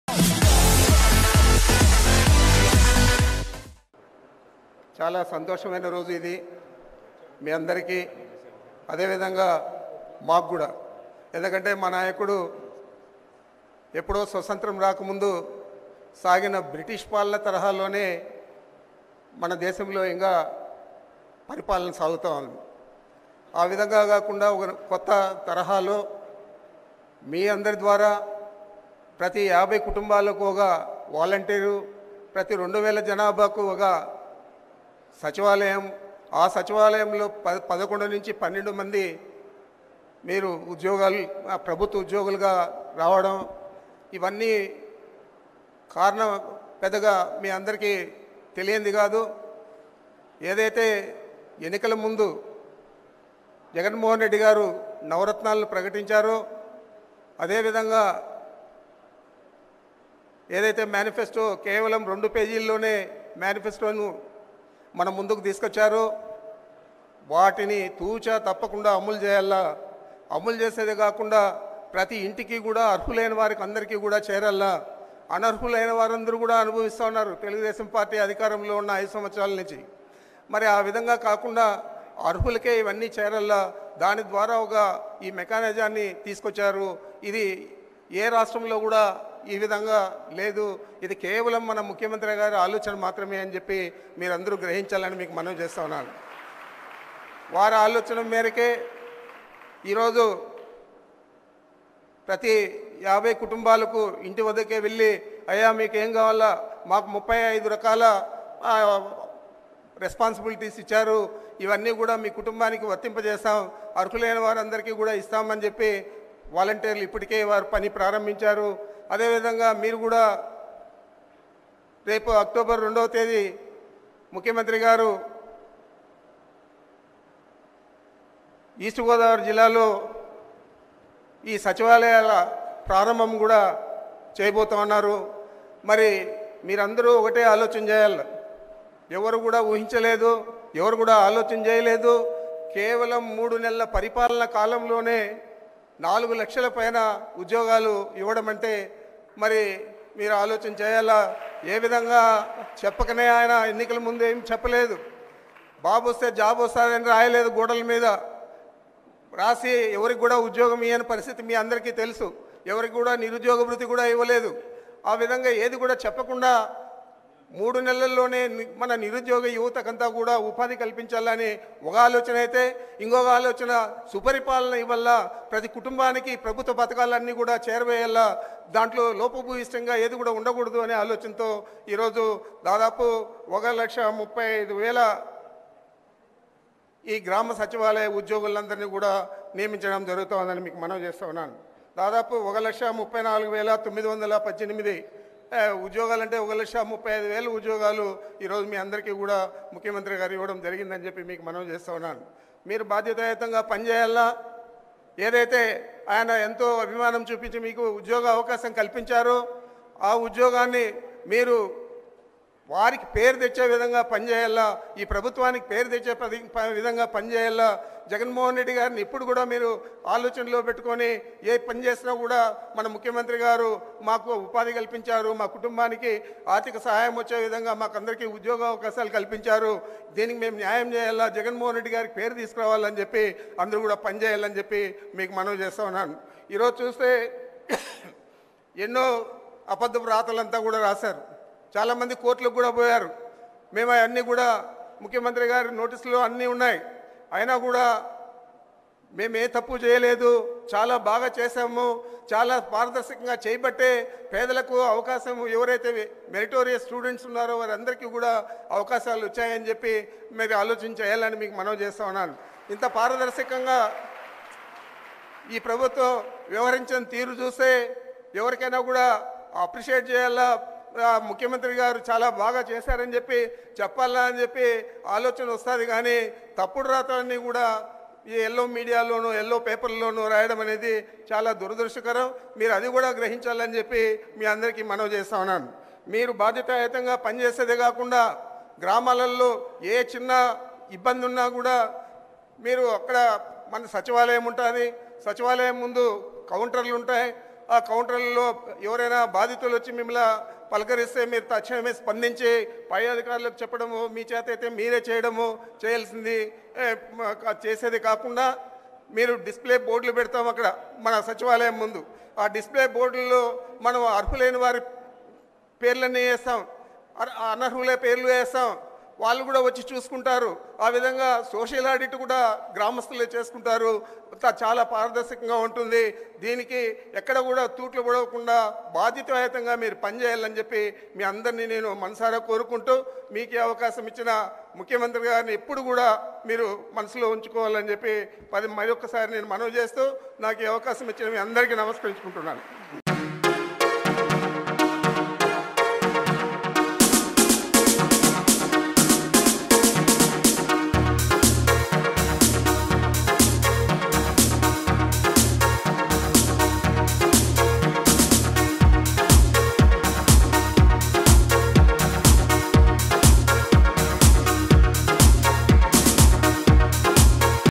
ఆ తప్పుడు రాతగాళ్లకు అవకాశం ఇవ్వొద్దు:మంత్రి పెద్దిరెడ్డి| Minister Peddireddy Slams Yellow Media
AP Miniter Peddireddy Ramachandra Reddy has made sensational comments on TDP Support Media.